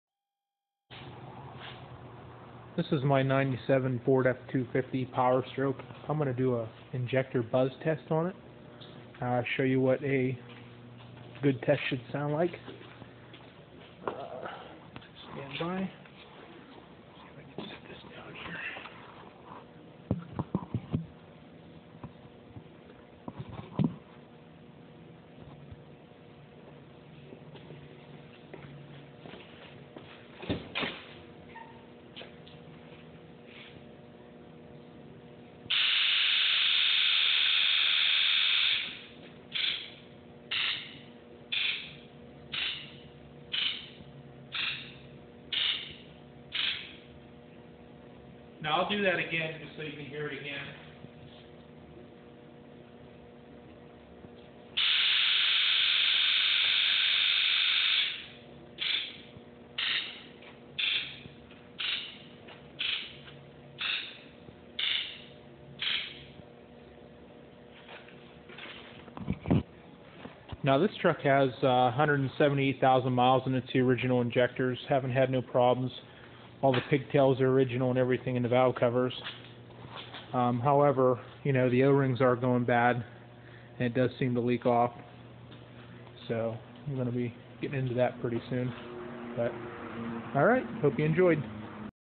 Powerstroke injector buzz test
injector buzz test on a 97 f250 powerstroke with a snapon modis